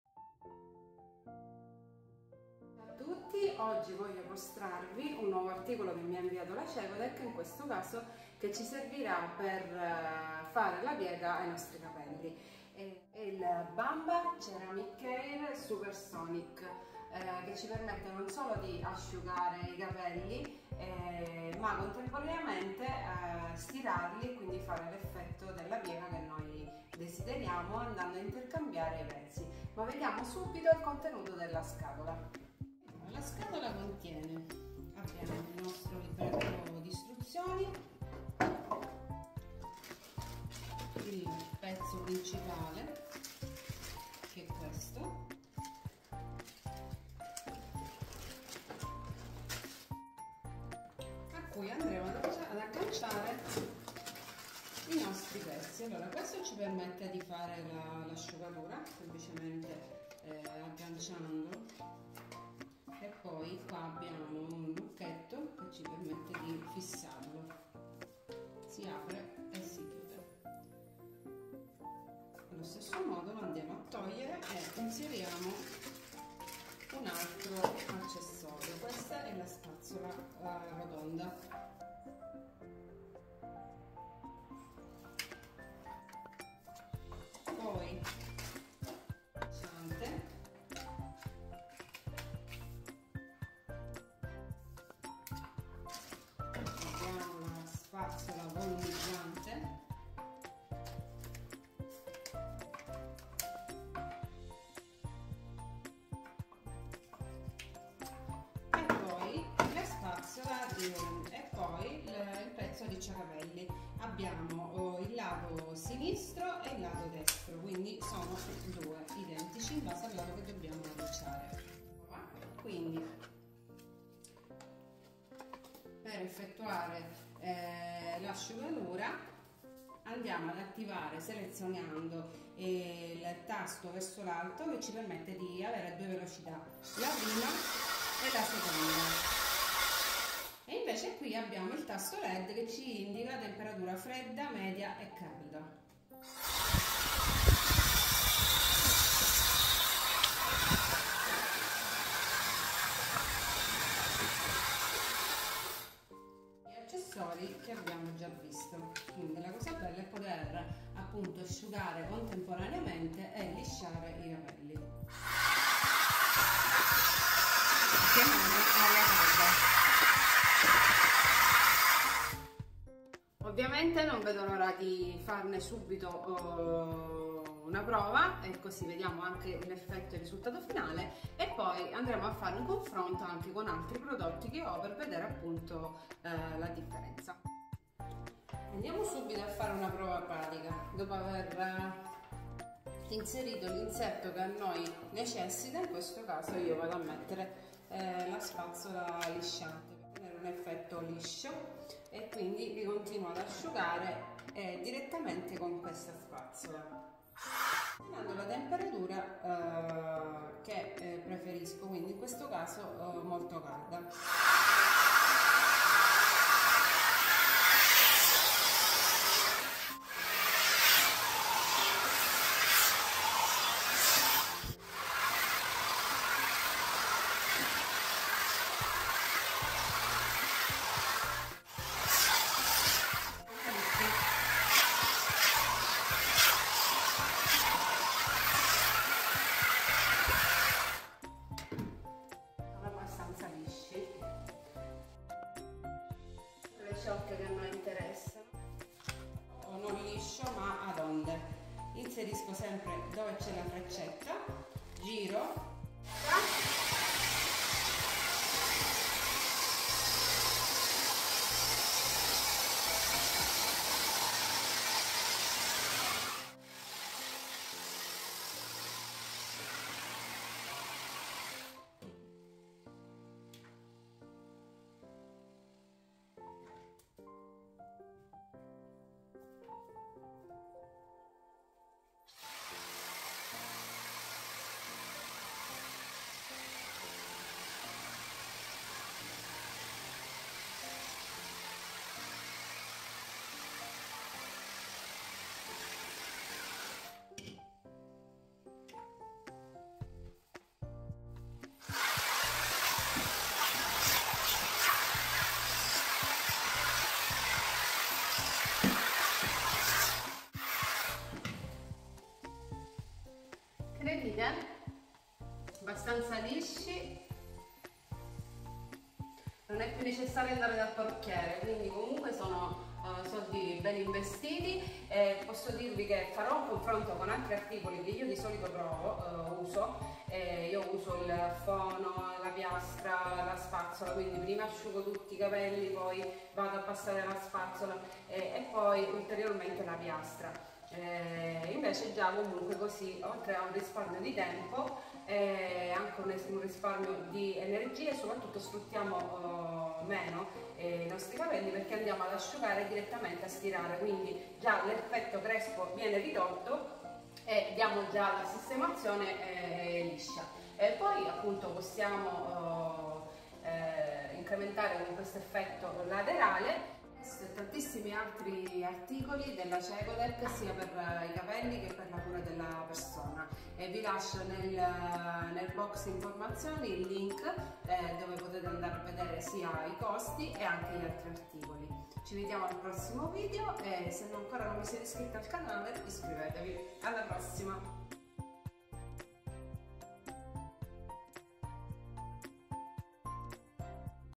Stop crespo grazie alla spazzola ad aria Bamba ceramicare SuperSonic della
Per me che ho tanti capelli,crespi e difficili da lavorare è stato un aiuto indispensabile che mi permette di evitare il parrucchiere quindi...𝐀𝐝𝐝𝐢𝐨 𝐜𝐚𝐩𝐞𝐥𝐥𝐢 𝐜𝐫𝐞𝐬𝐩𝐢  grazie alla spazzola ad aria Bamba ceramicare SuperSonic @cecotec.italia
che permette di ottenere una maggiore varietà di acconciature: asciugacapelli, spazzola lisciante, 2 cilindri arricciacapelli ,spazzola rotonda e volumizzante.

 ✅️la forza dell'aria avvolge i capelli attorno alla styler , creando ricci perfetti, senza dover fare nulla.

✅️Controlla la temperatura che applichi ai capelli grazie alle sue 3 impostazioni di temperatura.

✅️Ha 2 velocità del flusso d'aria per adattarsi a ogni acconciatura.

✅️Testine rivestite in ceramica, che forniscono un calore rapido e uniforme, che riduce i danni ai capelli.

✅️Ma la cosa davvero fantastica è che ha ioni negativi che neutralizzano gli ioni positivi presenti naturalmente nei capelli e provocano 𝗰𝗵𝗶𝘂𝘀𝘂𝗿𝗮 𝗱𝗲𝗹𝗹𝗮 𝗰𝘂𝘁𝗶𝗰𝗼𝗹𝗮, 𝘀𝗶𝗴𝗶𝗹𝗹𝗮𝗻𝗱𝗼 𝗹'𝘂𝗺𝗶𝗱𝗶𝘁à 𝗻𝗲𝗹 𝗳𝘂𝘀𝘁𝗼 𝗱𝗲𝗹 𝗰𝗮𝗽𝗲𝗹𝗹𝗼 𝗲 𝗿𝗶𝗱𝘂𝗰𝗲𝗻𝗱𝗼 𝗹'𝗲𝗳𝗳𝗲𝘁𝘁𝗼 𝗰𝗿𝗲𝘀𝗽𝗼

Questo è il link ler vedere ulteriori informazioni